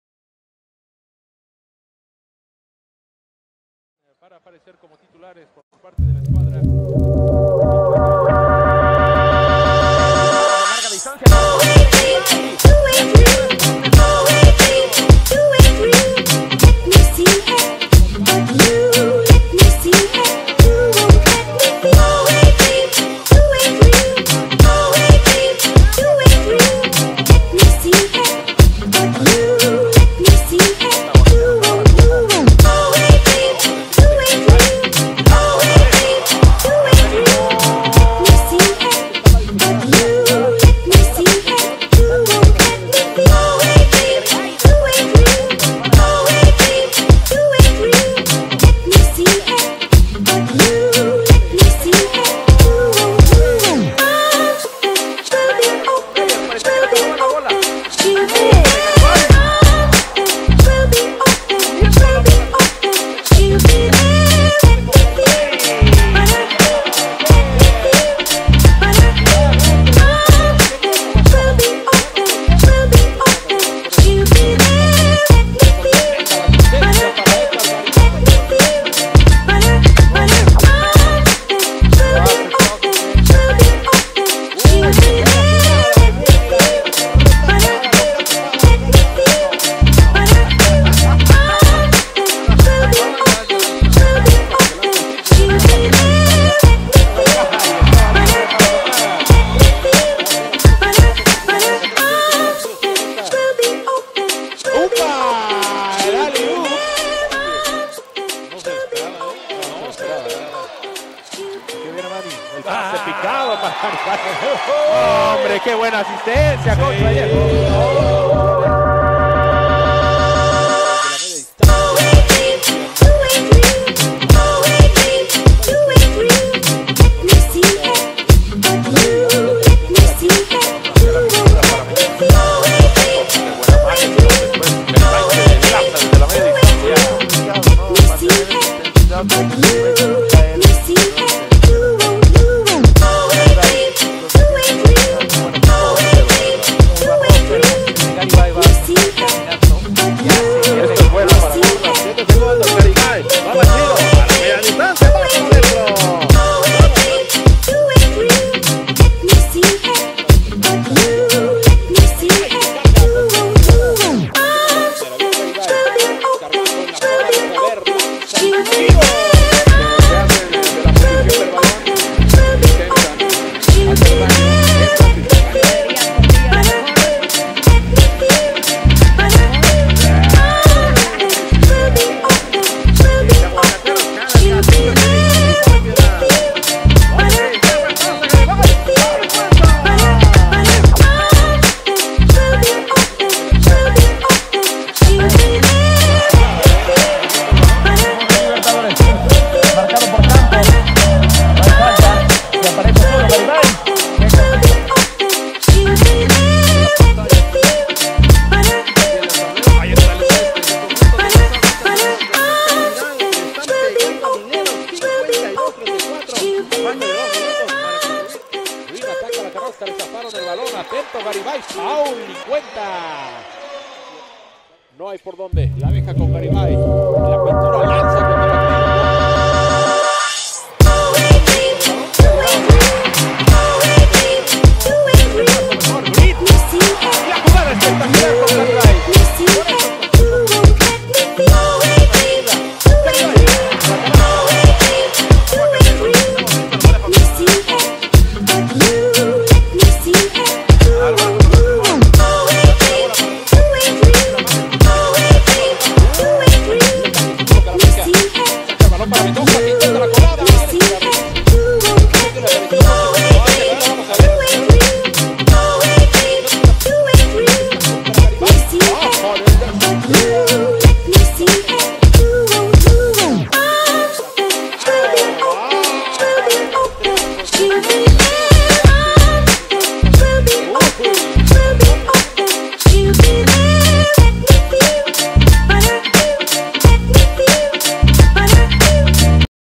Edgar Garibay 2019/2020 highlights DOES IT ALL!!
What a season for 6’10 international forward Edgar Garibay. Check out this tape of his 2019/2020 season with Aguacateros de Michoacan in Mexico's top league, LNBP, where Garibay does it ALL! Is there something this player can’t do!?